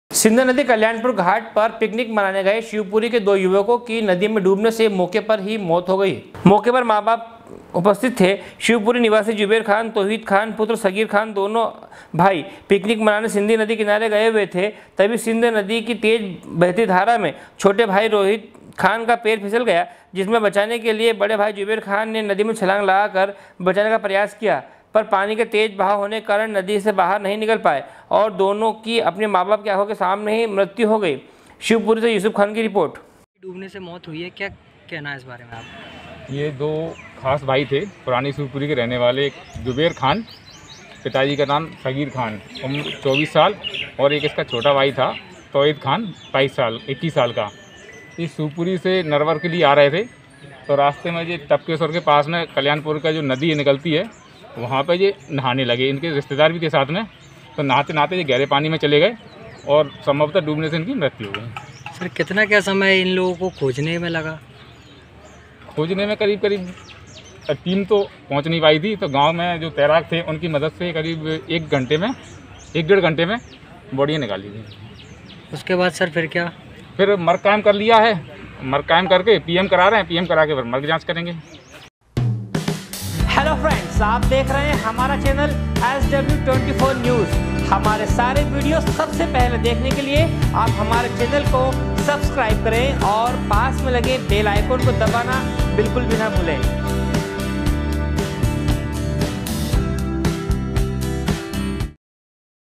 shivpuri- सिंध नदी कल्याणपुर घाट पर पिकनिक मनाने गए
shivpuri- SW 24 News Madhyapradesh shivpuri SW24news04_07_2021
सिंध नदी कल्याणपुर घाट पर पिकनिक मनाने गए शिवपुरी के दो युवकों की नदी में डुबने से मौके पर ही मृत्यू हो गई । शिवपुरी से युसूफ खान की रिपोर्ट